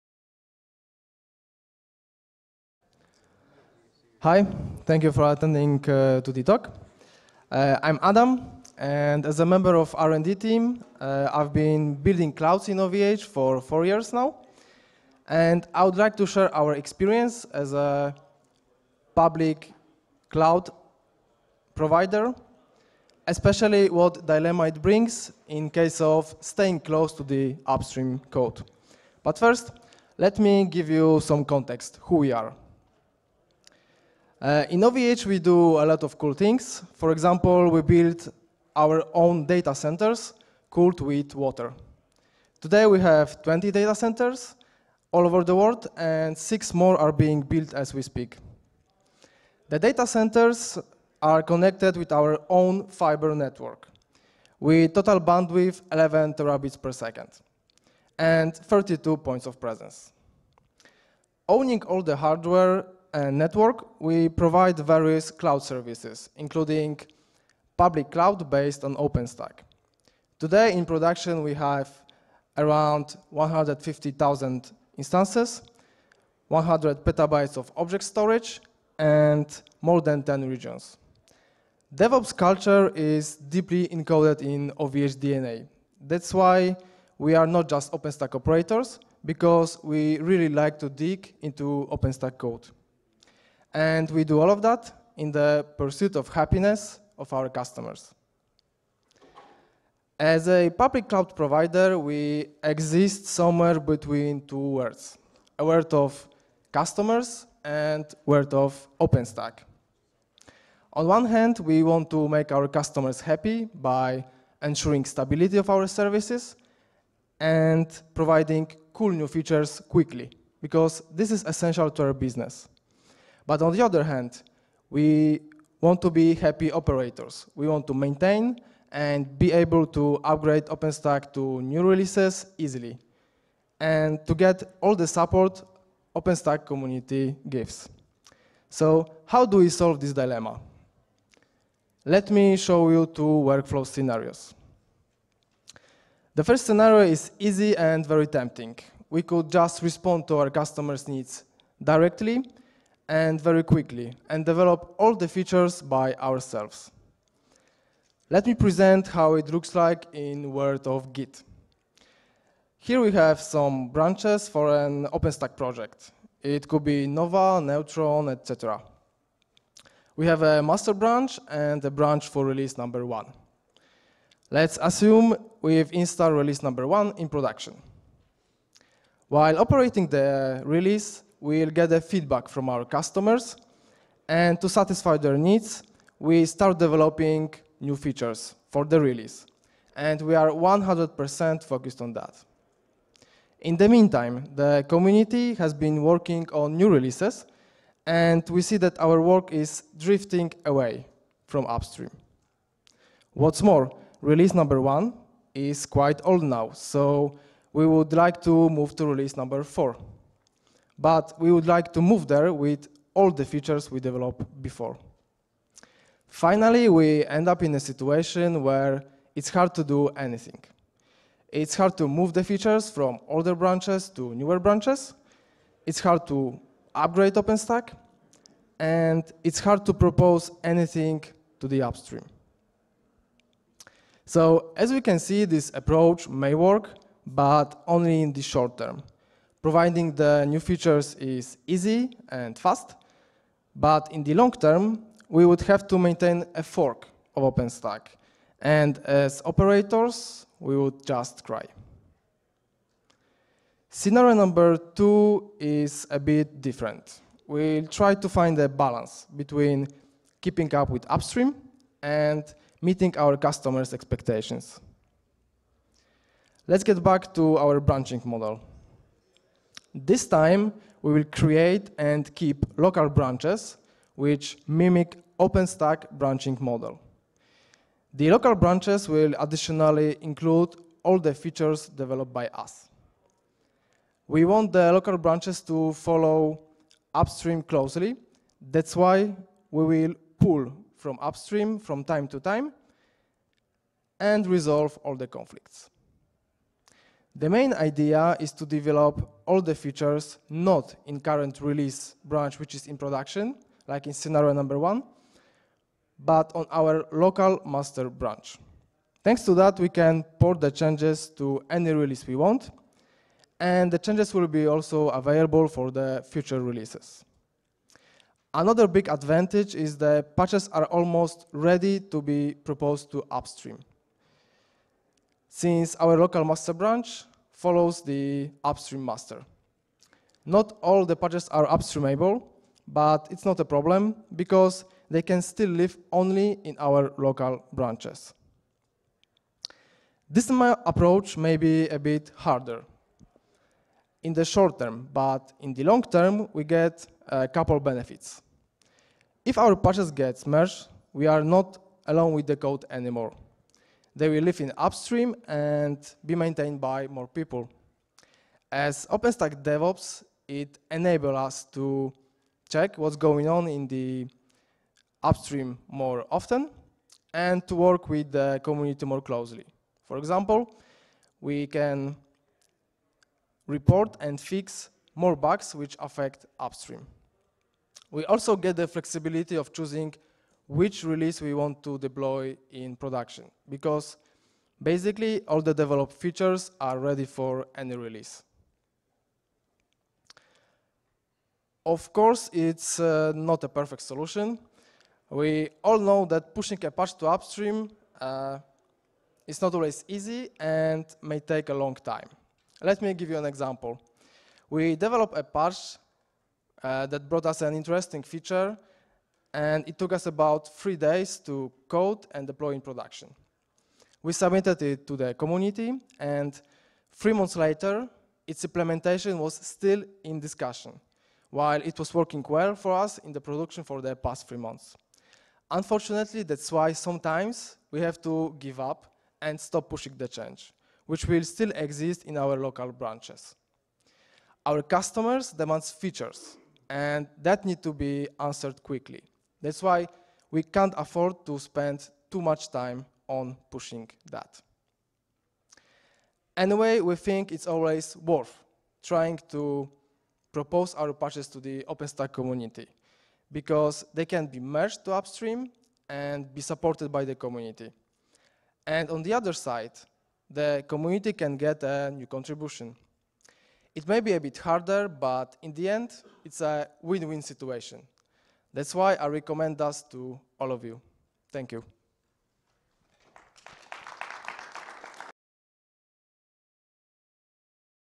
Public Cloud Dilemmas- Stay Close to the Upstream Code
As a public cloud provider, every day we face challenges in the pursuit of happiness of our customers.Choosing OpenStack as a base for our public cloud brings many advantages but may also be tricky.We want to share our journey, how we found a balance between keeping up with upstream, and meet our clients' expectations. On the first hand, fixing bugs and quickly developing new features for our infrastructure is very important for our Public Cloud offering. On the other hand, being in touch with u